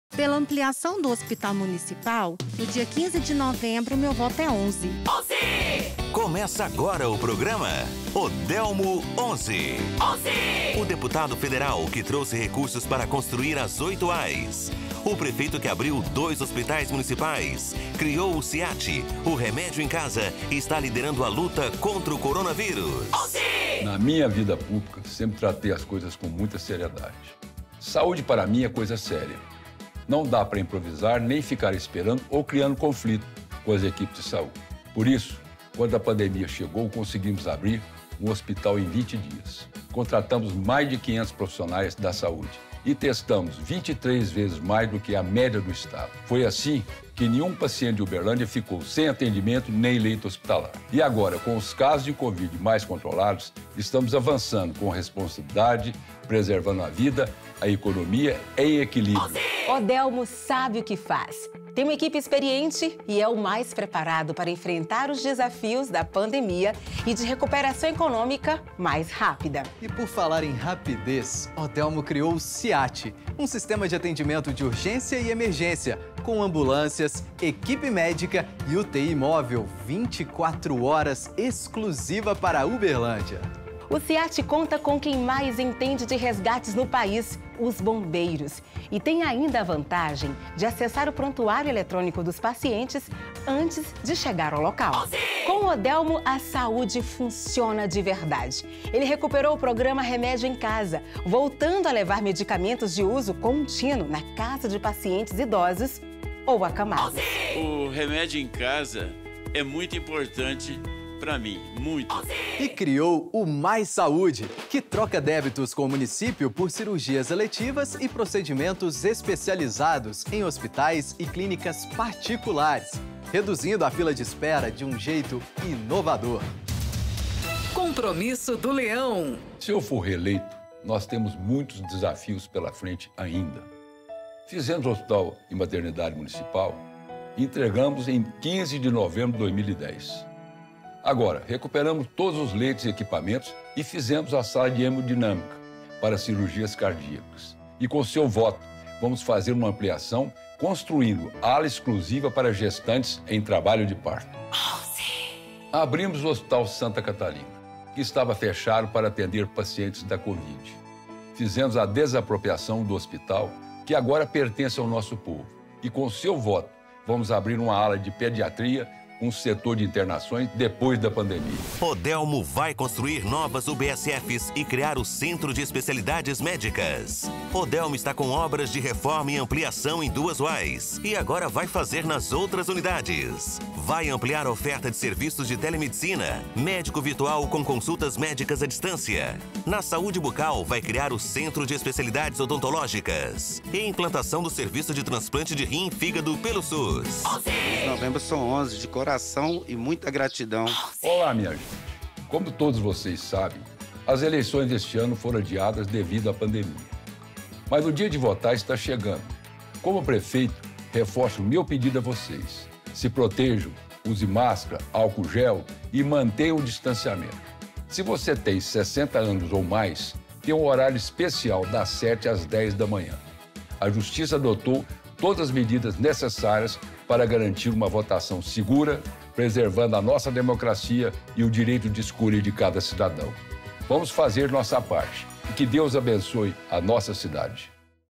Saúde - 06/11/2020
Saúde para mim é coisa séria! Assista neste programa todas as ações que fizemos para melhorar ainda mais este setor e também os nossos compromissos para, se eleito for, a próxima gestão.